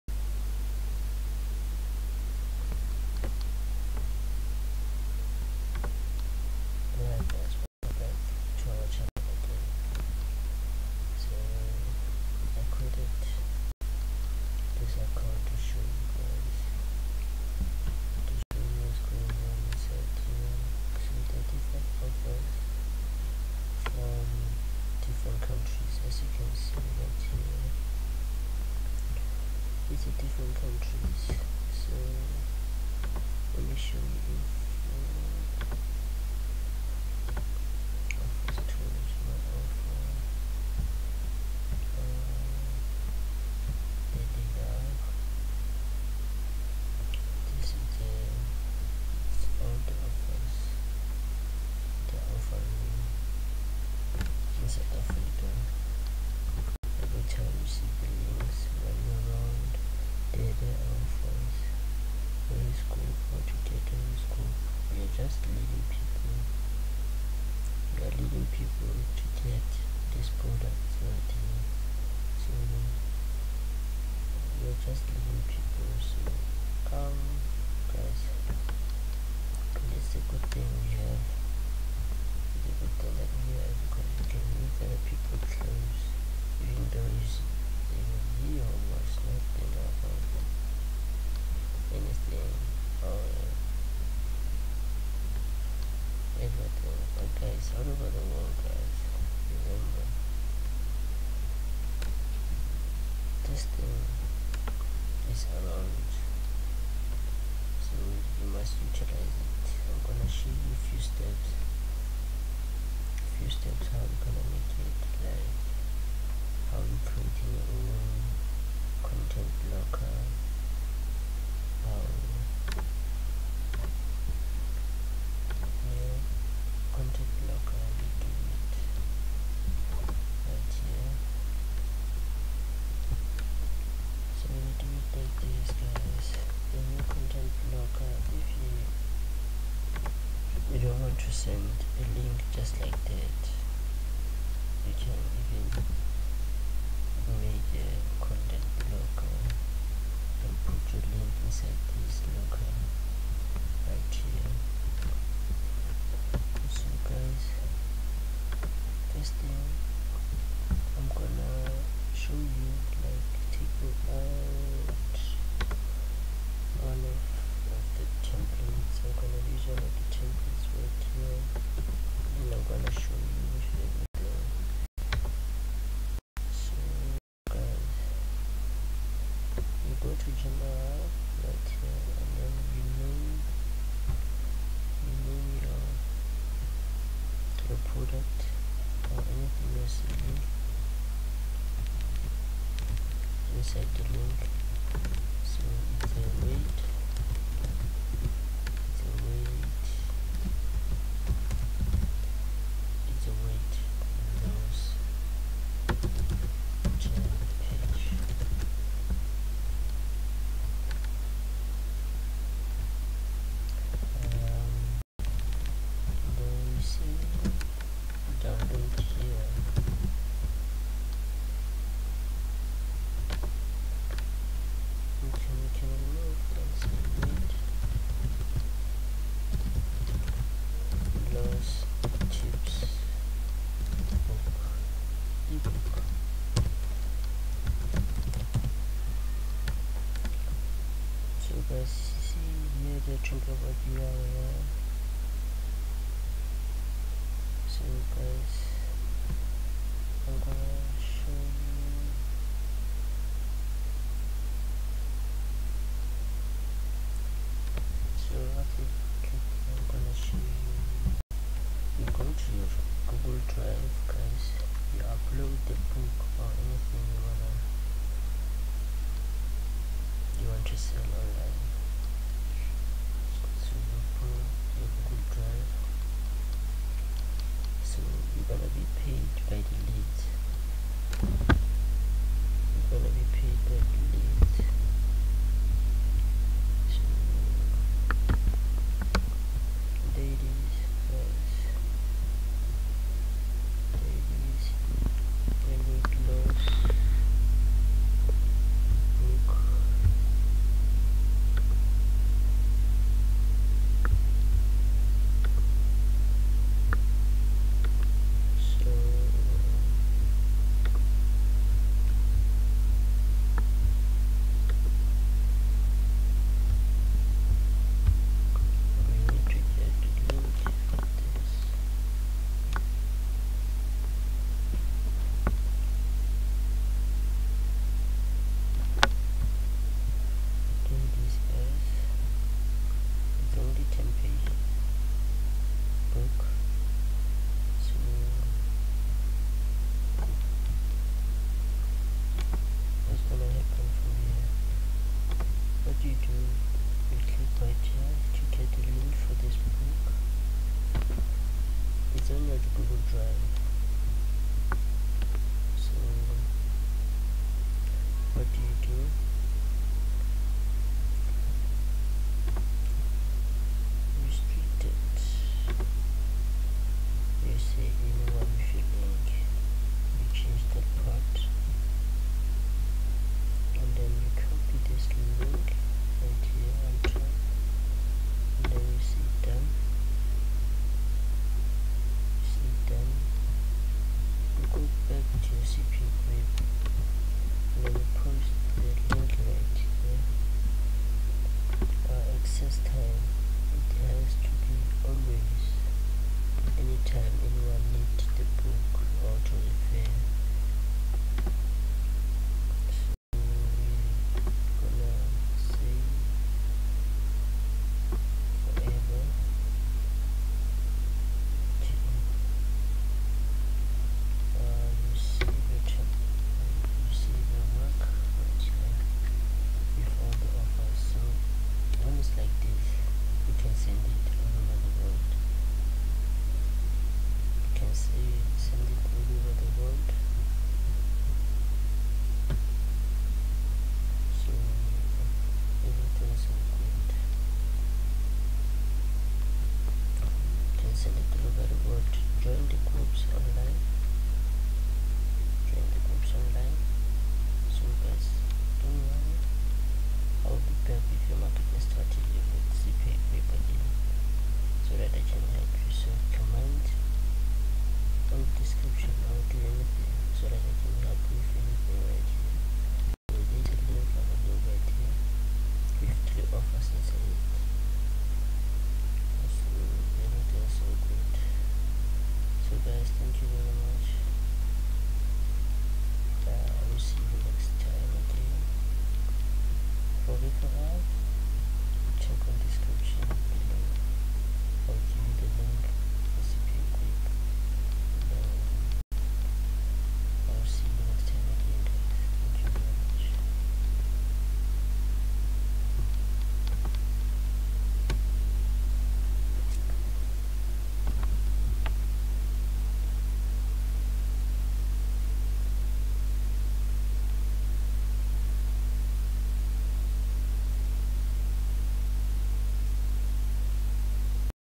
CPACRIP TUTORIAL - How To Use Cpagrip -How To Make Money With CPAGRIP
CPAGRIP Marketing For Beginners | How To Make Money With CPAGRIP Tutorial

What's up, guys? In this tutorial I am going to breakdown for you, how to earn money with cpa offers on cpagrip.com. I am going show you how to sign up on cpagrip, how to find the best offers and how to make the most money with them.